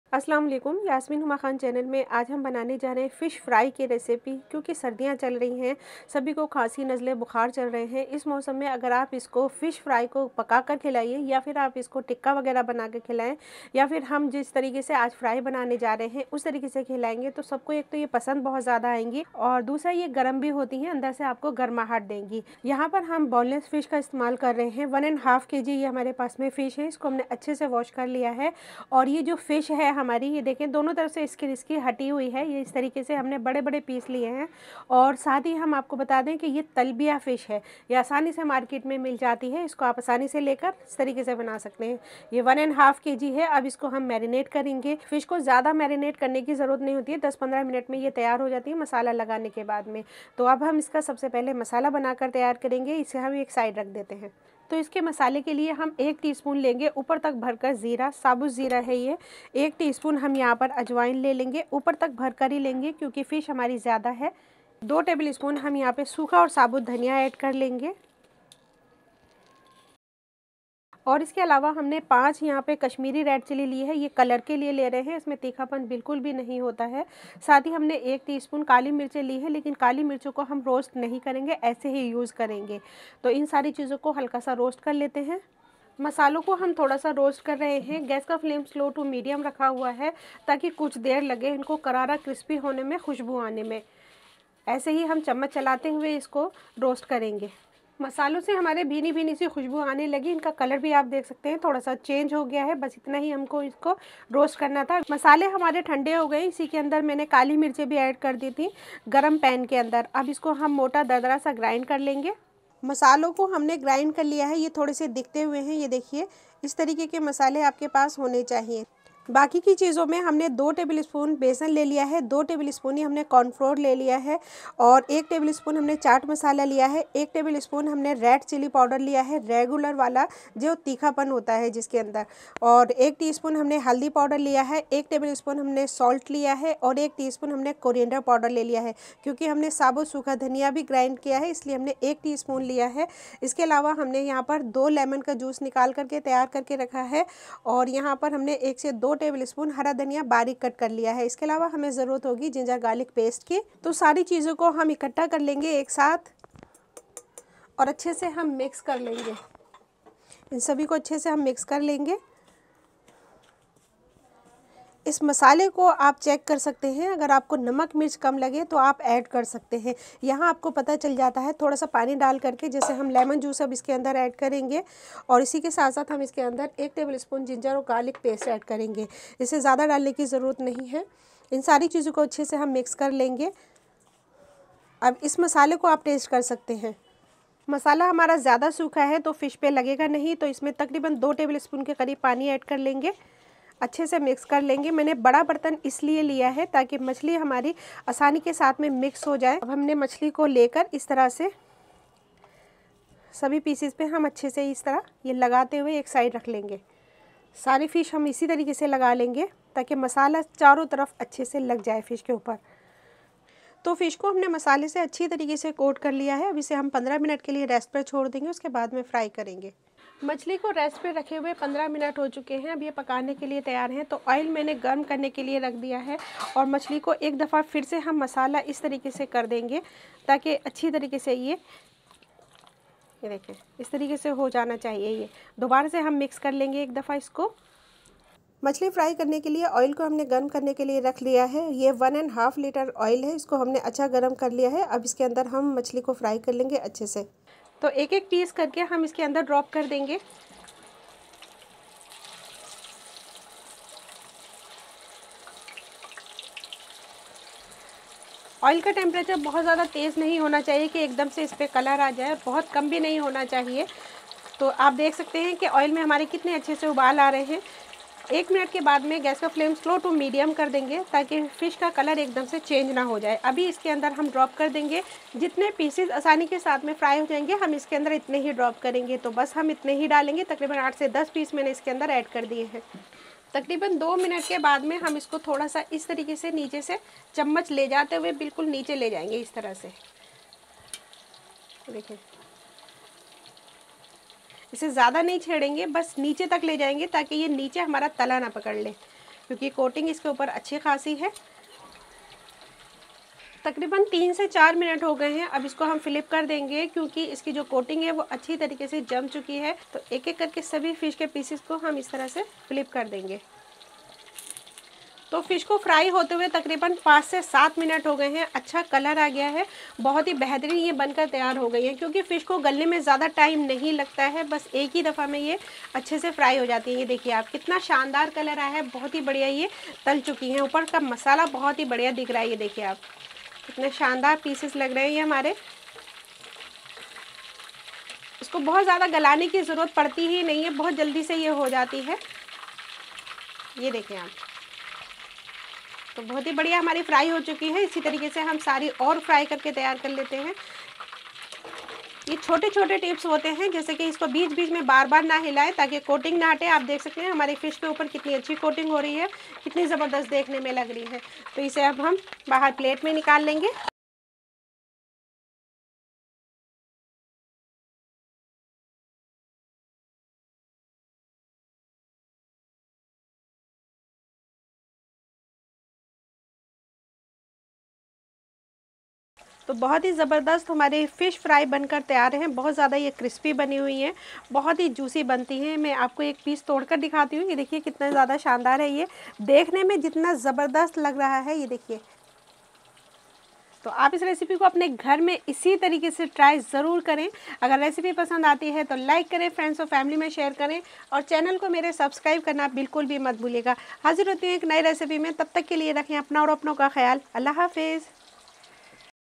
FISH FRY Juicy & Crispy | Restaurant se ziyada delicious Fish Fry ab ghar par hi banaye 💖
Aaj ki bht zabardast recipe Fish Fry ki Hai, super juicy & tender fish fry banaiye bht bht asaani  se apne ghr par, Restaurant se bhi ziyada tasty aur delicious fish fry ab ghar par hi banaye

(Ingredients)

For Making,
Boneless Fish 1.5 kg.
Sabud Dhaniya 2 tbsp.
Kashmiri Lal Mirch 5.
Kali Mirch 1 tsp.
Lal Mirch Powder 1 tbsp.
Haldi Powder 1 tsp.
Namak 1 tbsp.
Dhaniya Powder 1 tsp.
Nibu juice 2.
Hara Dhaniya 1 tbsp.